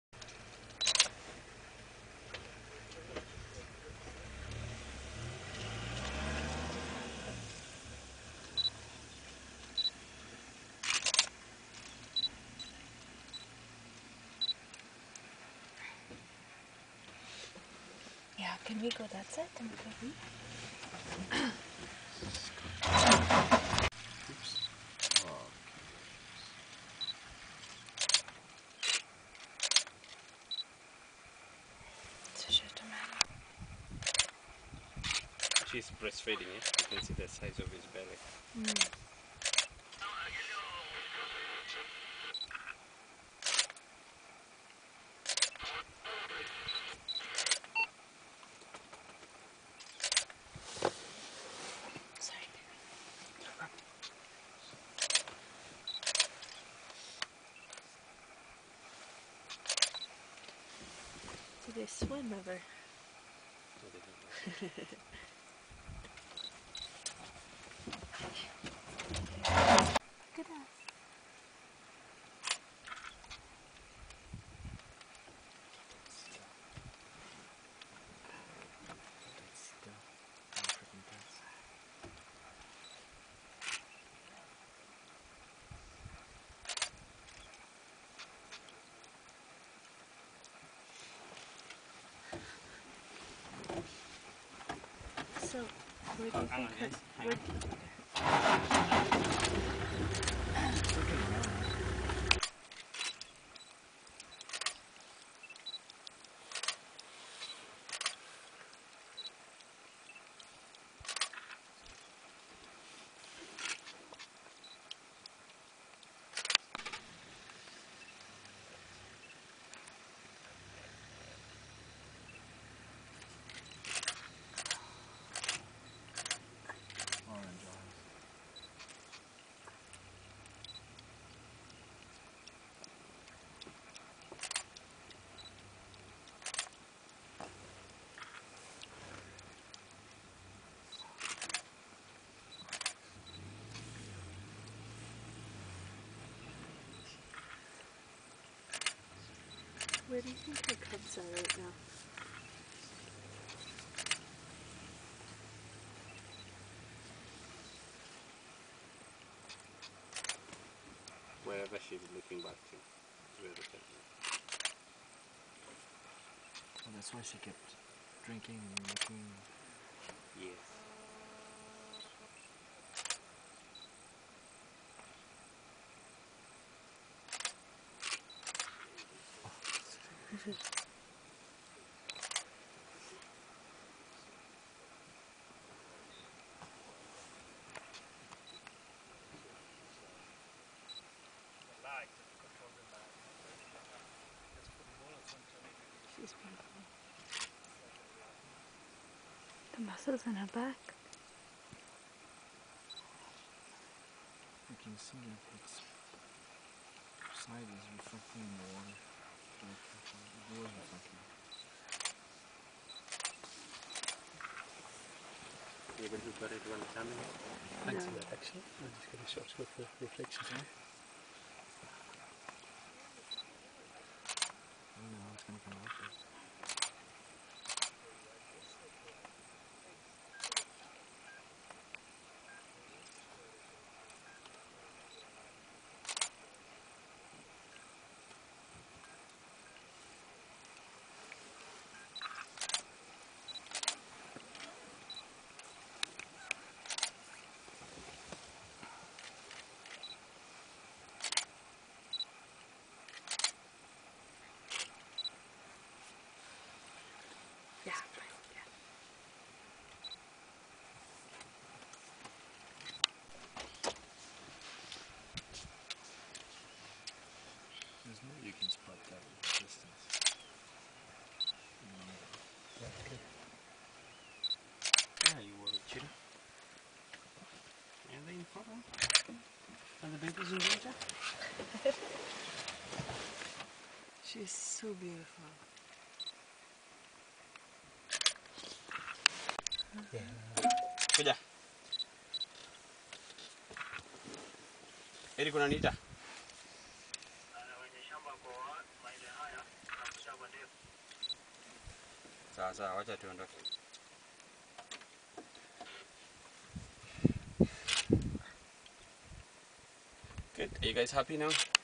Cheetah drinks from a waterhole in the Masai Mara
A beautiful mother cheetah has a drink from a waterhole in the Masai Mara. We had heard that she had two cubs in the area, which she had hidden as she went for a drink of water. Coming back that way later in the day, we saw the reason for her vigilance. Two big male lions were lying in some shade at the top of the hill.  But the cubs were safe and were seen with their mother a few days later.